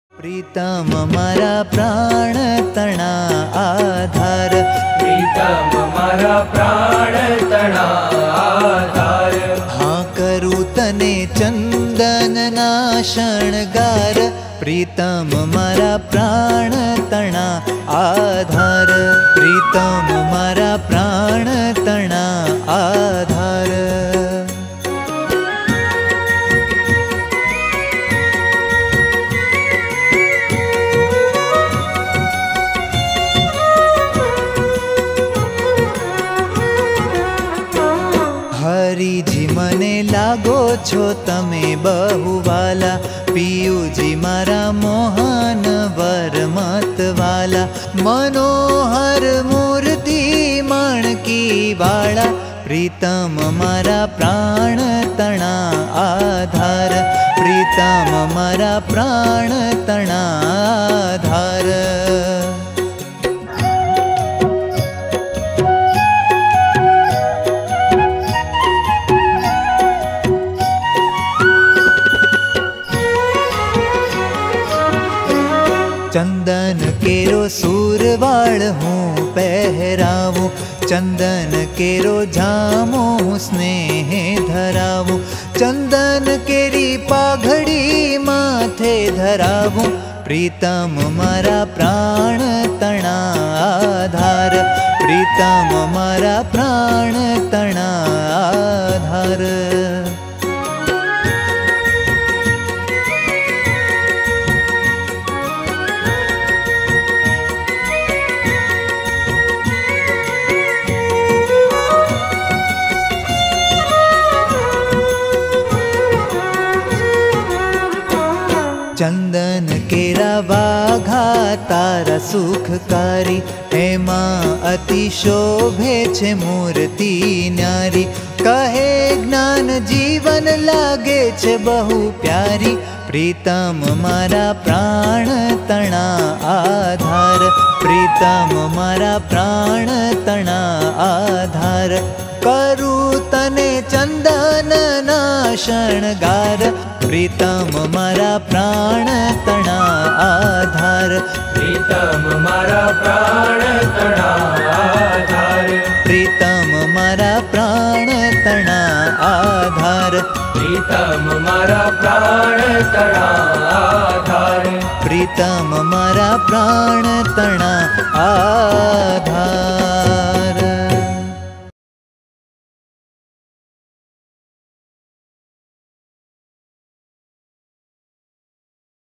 Chandanvaghama ShriHari | ચંદનવાઘામાં શ્રીહરિ | Harikrushna Maharaj | Daily Darshan | 22 May 2021
Track Name: Harikrushna Maharaj | Chandanvaghama ShriHari
Guj. Track Name: હરિકૃષ્ણ મહારાજ | ચંદનવાઘામાં શ્રીહરિ
Type: Other
Category: Daily Darshan
Event Date: 22 May 2021

પ્રેરક: Pu. Gyanjivandasji Swami - Kundaldham

Playlist Name: Chandan Vaghama ShriHari | ચંદન વાઘામાં શ્રીહરિ | Harikrushna Maharaj-Tapu Maharaj | હરિકૃષ્ણ મહારાજ - ટપુ મહારાજ | By Pu. Gyanjivandasji Swami - Kundal